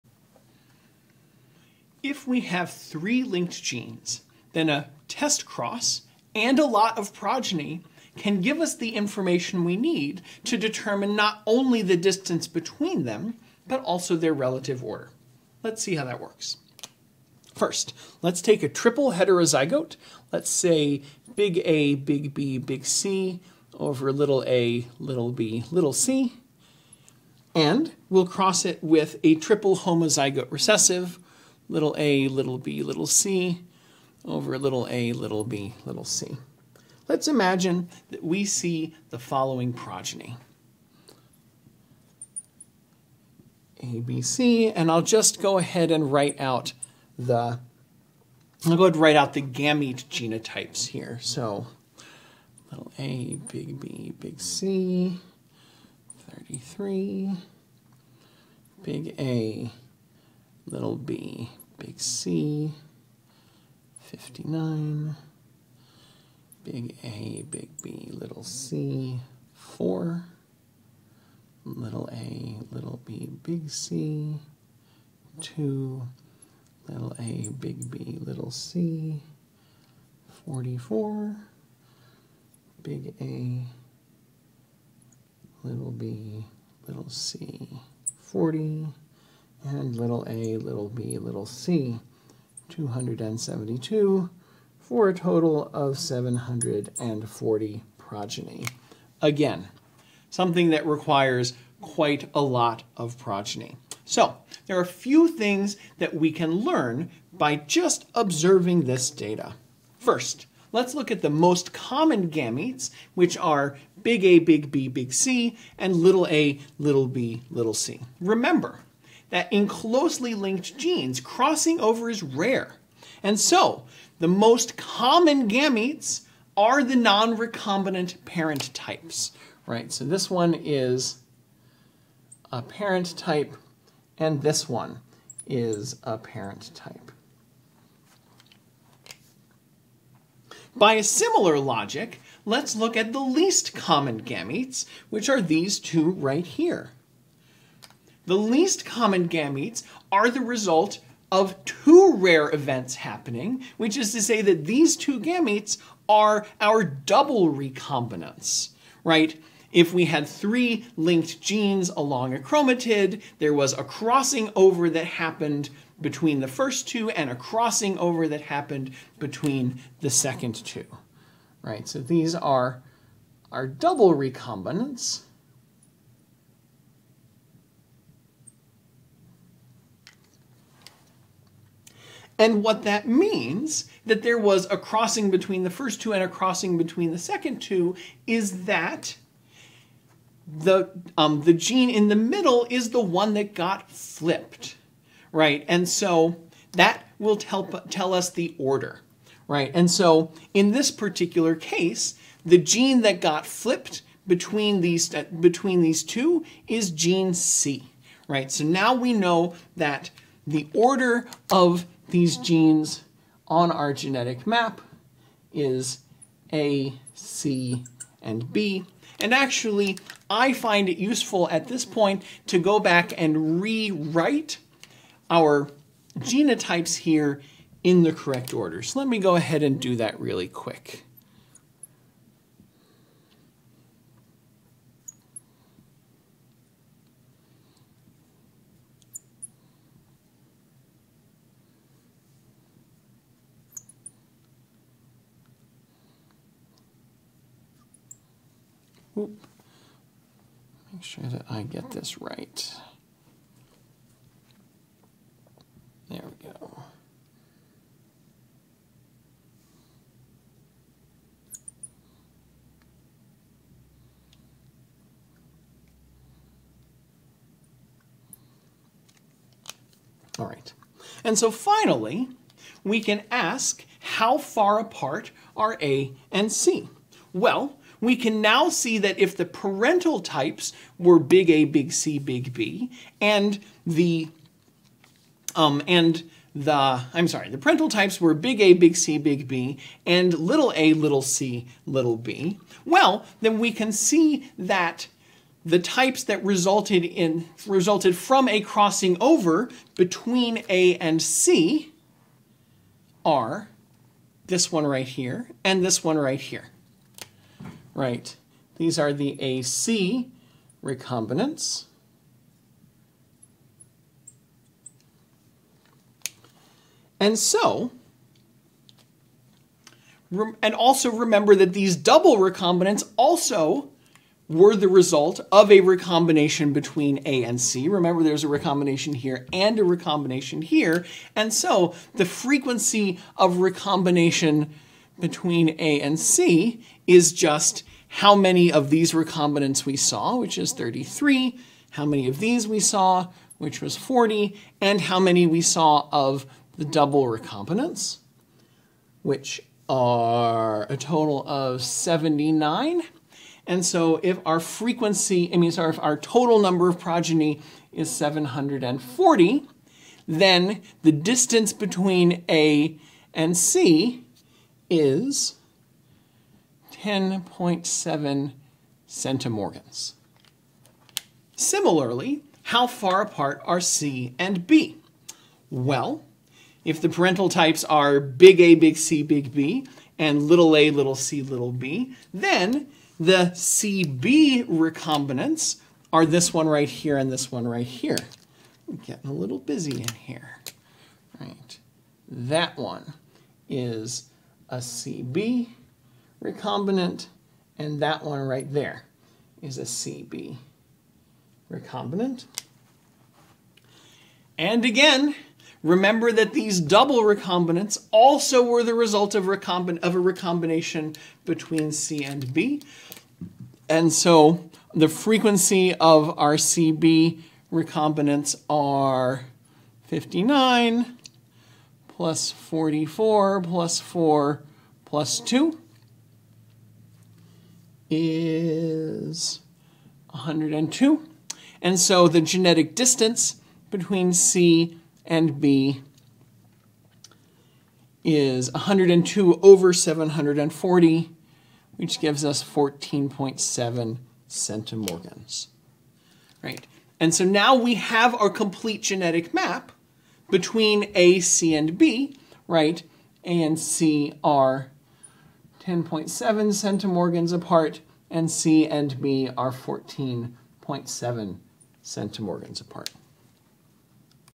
Mapping genes with a three point testcross
How do we determine the order and distance between three genes using a three-point test-cross? How do we determine the parent types and double-recombinant types? How do we determine the map distance between each pair of genes?

This video lecture was recorded at the University of Wisconsin -- Stout in the fall of 2021. It accompanies my BIO 332 Genetics course, which was taught with Hartl's "Genetics: Analysis of Genes and Genomes."